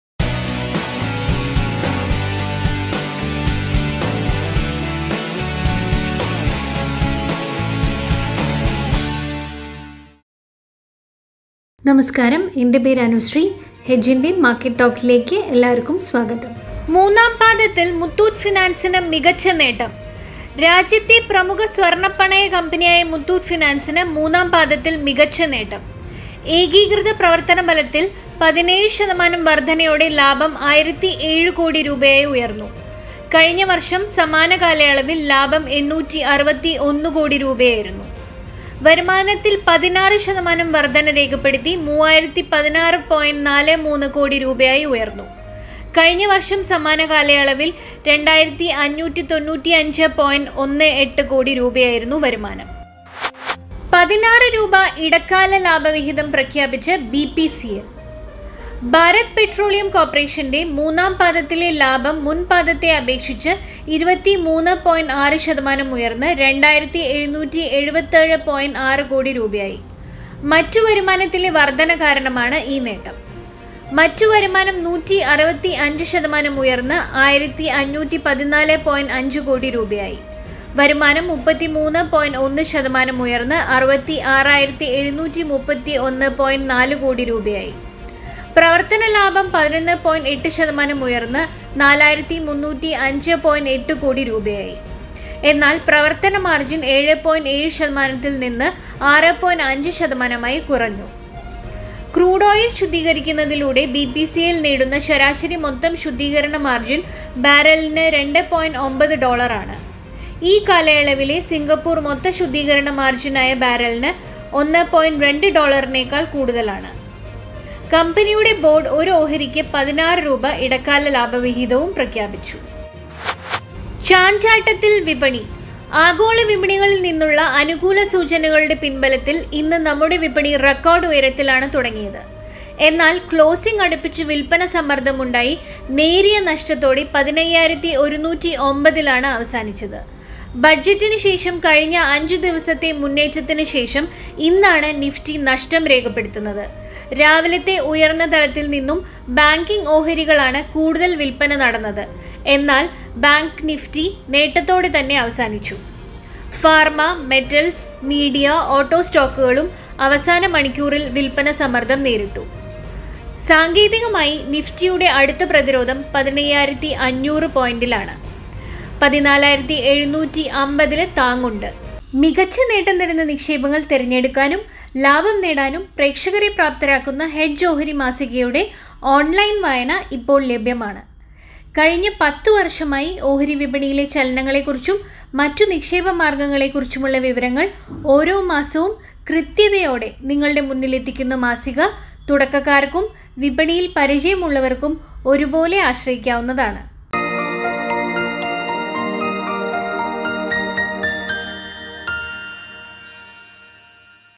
മൂന്നാം പാദത്തിൽ മുത്തൂറ്റ് ഫൈനാൻസിനു മികച്ച നേട്ടം | Hedge Market Talk 9th February 2021
രാജ്യത്തെ പ്രമുഖ സ്വർണ പണയ കമ്പനിയായ മുത്തൂറ്റ് ഫൈനാൻസിനു മൂന്നാം പാദത്തിൽ മികച്ച നേട്ടം. ഏകീകൃത  പ്രവർത്തന ഫലത്തിൽ 17 ശതമാനം വർധനയോടെ ലാഭം 1,007 കോടി  രൂപയായി ഉയർന്നു കഴിഞ്ഞ വർഷം സമാന കാലയളവിൽ ലാഭം 861 കോടി രൂപയായിരുന്നു. വരുമാനത്തിൽ 16 ശതമാനം വർധന രേഖപ്പെടുത്തി 3016.43 കോടി  രൂപയായി ഉയർന്നു കഴിഞ്ഞ വർഷം സമാന കാലയളവിൽ 2595.18 കോടി രൂപയായിരുന്നു വരുമാനം.